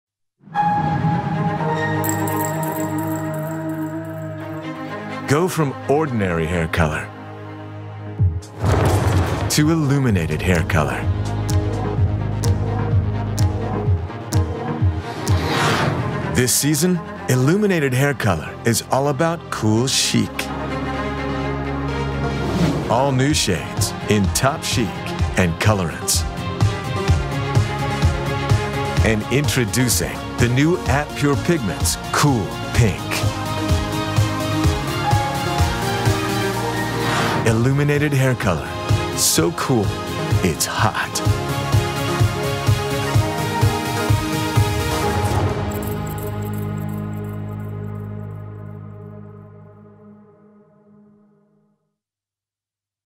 Goldwell Elumenated Hair Color | The Art & Science of Direct Dye Mastery | Goldwell Education Plus
Go from ordinary hair color to Elumenated Hair Color. This season Elumenated Hair Color is all about Cool Chic. Experience all new shades in Topchic and Colorance and the new @Pure Pigments Cool Pink. Elumenated Hair Color - so cool, it's hot.

TOPCHIC NEW @ELUMENATED SHADES
With outstanding cool shine.
- 9N@Pk
inspired by SAKURA PINK
- 7BP@Pk
inspired by RICH TEA
- 6NN@CV
inspired by COCO DE MER

COLORANCE EXPRESS TONING @ELUMENATED SHADES
For a fast 5 min refinement and toning opportunities after a lightening or highlift service.
- @Elumenated Lavender
inspired by LILATECH
- @Elumenated Beige Mint
inspired by NEO MINT
- @Elumenated Rosé

@PURE PIGMENTS NEW SHADE COOL PINK
Go beyond cool and reinvent any Topchic or Colorance color with a mesmerizing cool pink tone.
Inspired by CASSIS

THE ART AND SCIENCE OF DIRECT DYE MASTERY
Imagine the most stunning colors. And now imagine reflections of brilliant light in a full spectrum of vibrant colors. Elumenation is the art of adding extraordinary color brilliance and shine to every color.

IT´S A CLASS OF ITS OWN
Some innovations are too big for just one product. With Elumenation we created a new category of Elumenated Hair Color. After two decades of research and development with Direct Dyes, Goldwell has created colors with amazing capabilities – which change the way we see color forever.